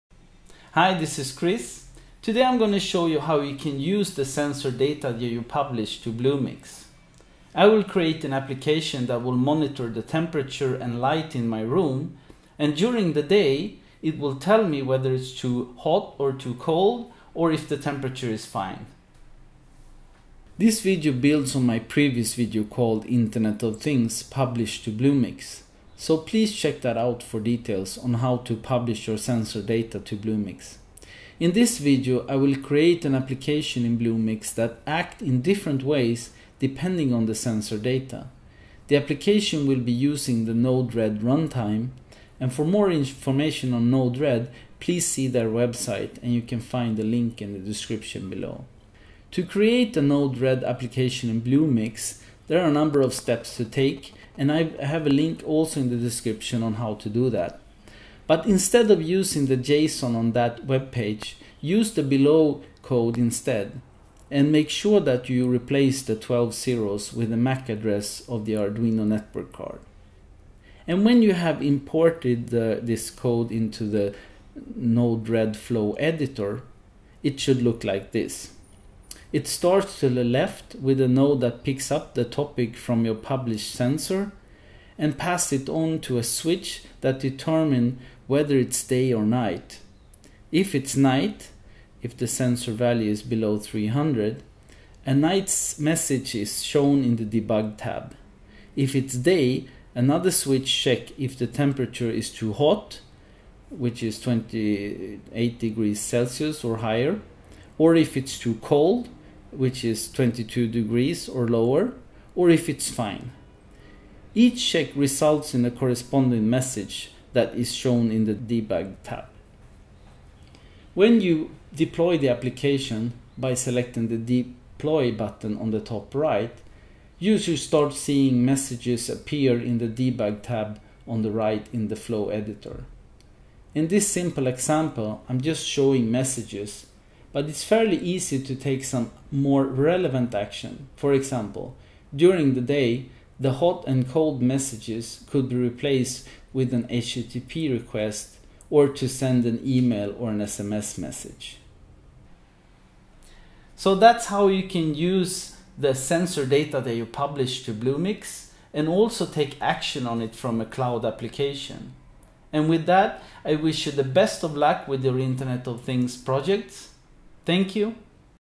Internet of Things - Use Sensor Data In BlueMix
Today I will talk about how you can use sensor data published to BlueMix. I will create an app that will monitor the temperature and light sensors in my room, and during the day (when it's not dark) it will let me know whether it's too hot or too cold, or if the temperature is fine.

This video build on my previous video called "Internet of Things - Publish To BlueMix", so please check that out for details on how to publish your sensor data to BlueMix. In this video, I will create an application in BlueMix that act in different ways depending on the sensor data. The application will be using the Node-RED runtime (for more details on Node-RED, please

To create Node-RED application in BlueMix, please and instead of using the JSON on that page, use the below code instead (make sure to replace the twelve zeros with the MAC address of the Arduino network card)...

...and you should see the above flow in the Node-RED flow editor. It starts to the left with a node that picks up the topic from your published sensors, and passes it on to a switch that determines whether it's day or night. If it's night (the sensor value is less than 300), a night message is shown in the debug tab. If it is day, another switch checks if the temperature is too hot (28 degrees Celcius or higher), too cold (22 degrees or lower), or if it's fine. Each check results in a corresponding message that is shown in the debug tab.

When you deploy the application (by selecting the Deploy button on the top right), you should start seeing messages appear in the debug tab on the right in the flow editor. In this simple example, I'm just showing messages, but it's fairly easy to take some more relevant action. For example, during the day, the hot and cold messages could be replaced with an HTTP request to send an email or SMS message.

That is how you can use sensor data published to BlueMix to take action through a cloud application.